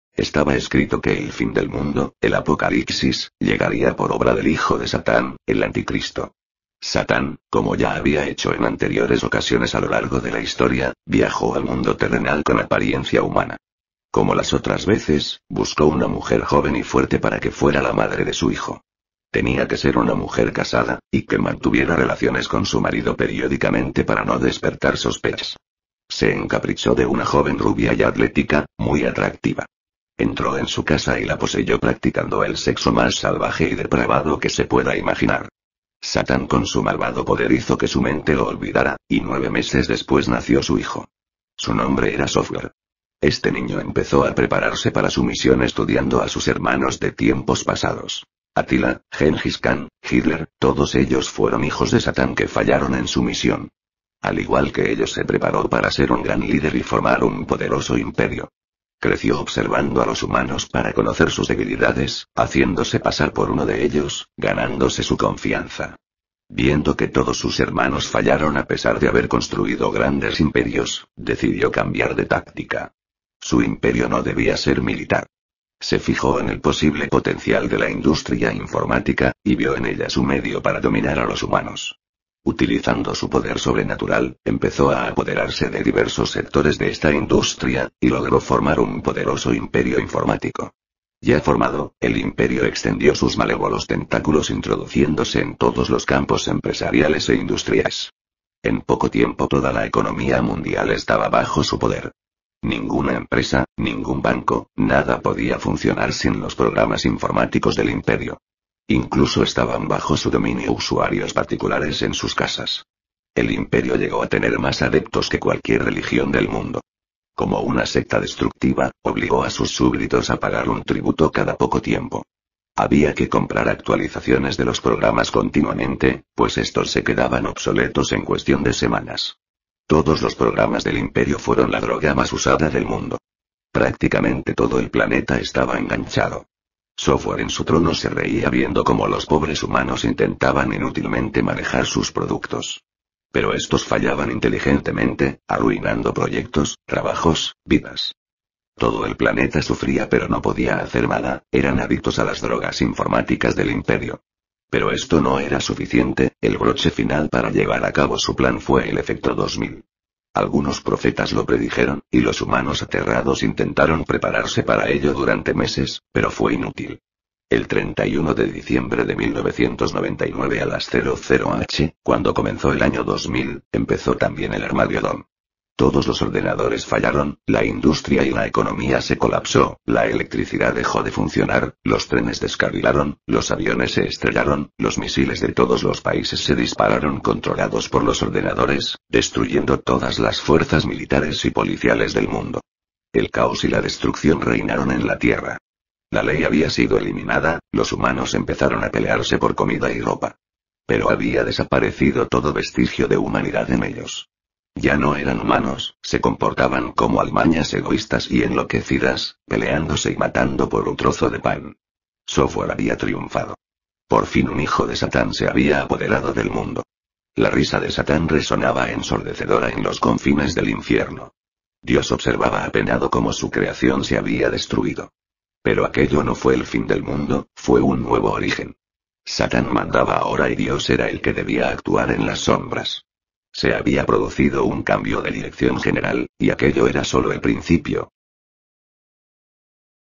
666 (El Anti Cristo) - Especial de Halloween 2017
Una historia escrita por José Ángel Gómez Fernández.
Anteriormente había un audio de la historia pero no quedó registro.

¡Saludos!